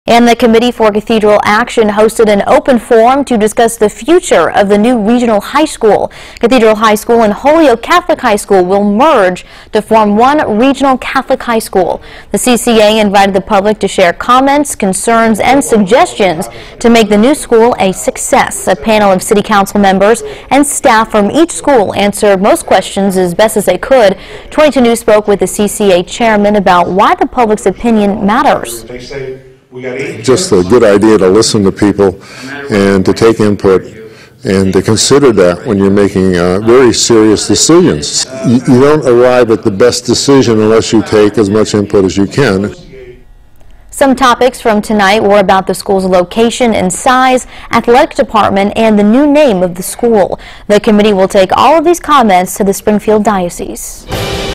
CCA hosts open forum discussing regional Catholic high school
The Committee for Cathedral Action hosted an open forum to discuss the future of the new regional high school